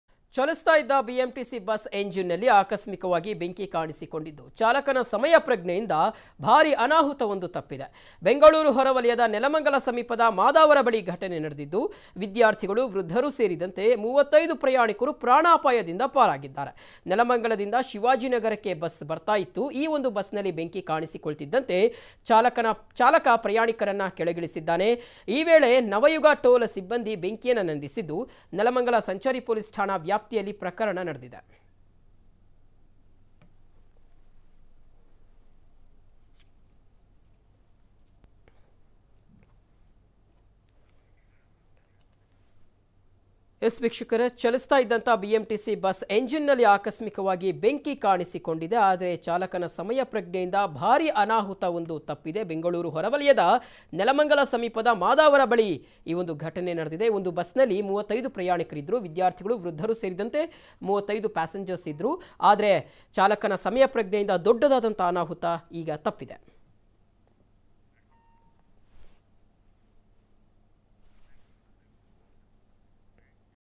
BMTC Bus Not Safe? Bus Catches Fire Near Nelamangala | TV5 Kannada
BMTC Bus caught fire in Madavara near Nelamangala.